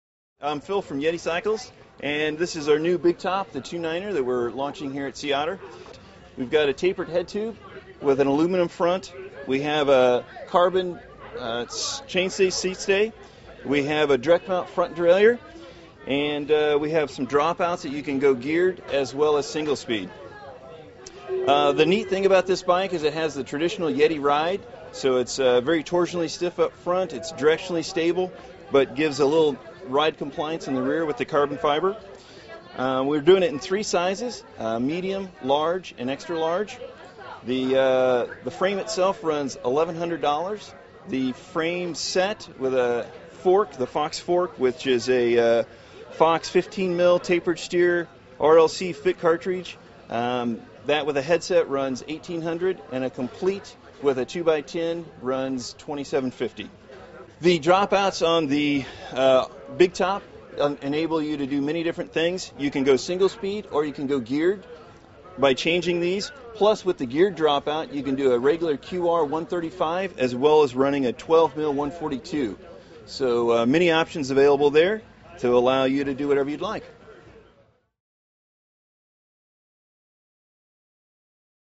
Yeti Big Top 29er Hardtail
Check out Yeti's Big Top 29er hard tail mountain bike from the 2010 Sea Otter Classic race/expo.